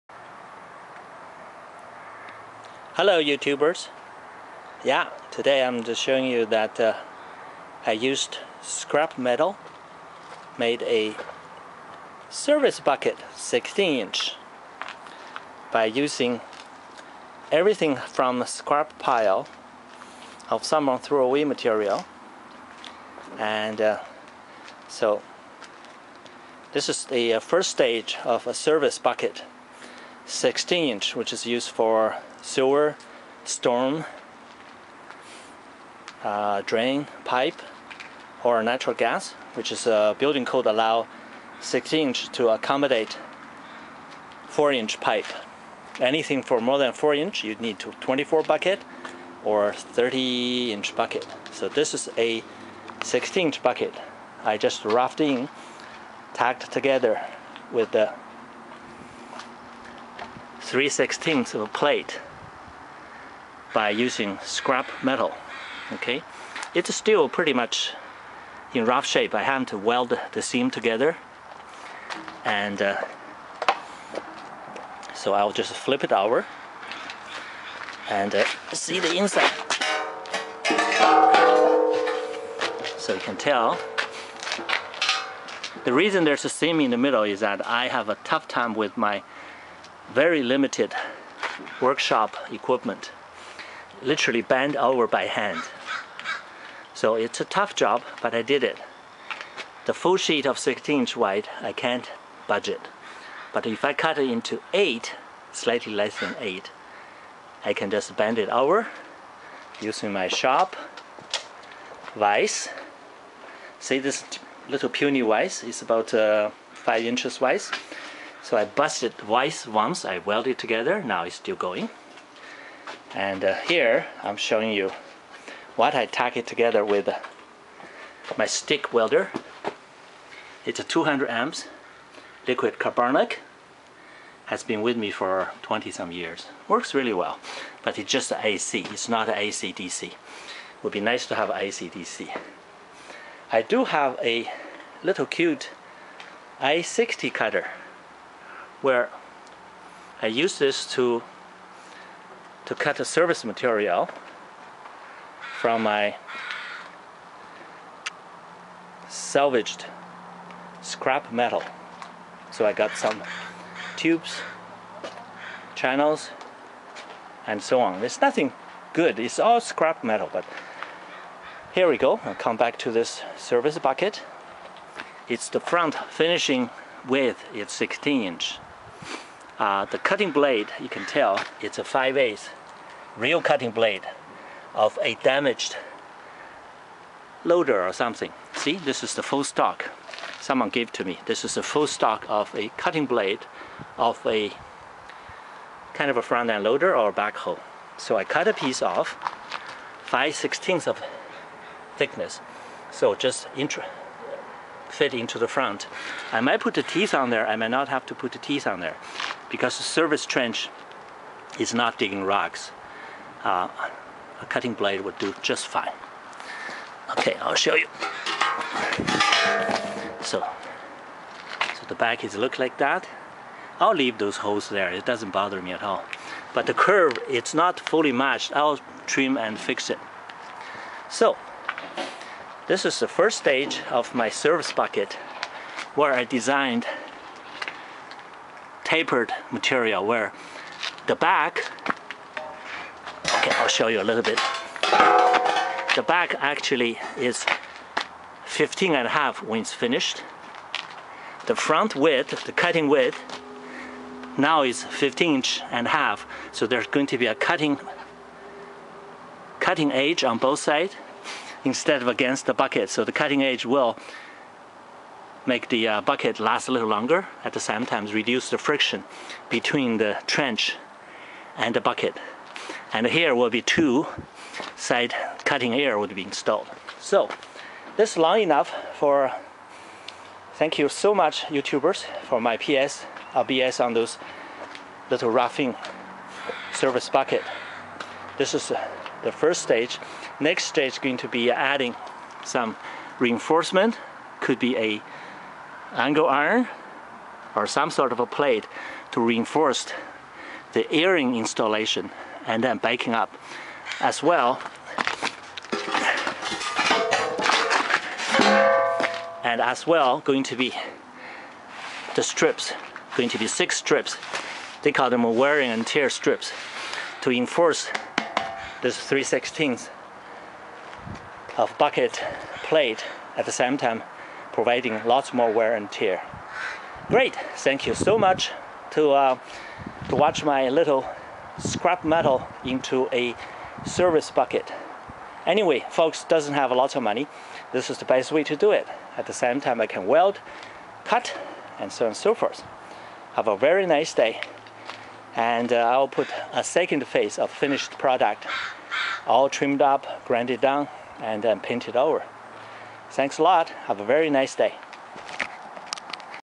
How to make a 16" excavator bucket from scrape metals (1)
I have use some scape 3/16 plate to fabricate a service bucket for my Hitachi EX50 excavator.  It is a 16" 5/8 " cutting blade design.  It is just tucked together to see how it looks and on next video, I will install pin ears  enforcing channel and a pair pin ear with 1/2 plates and pin backing. Also double the side cutting edges as well as corner cutting tips  and buttom wearing strips.  There is fair bit of wielding, so I am getting another box of rods 7014 or 6013.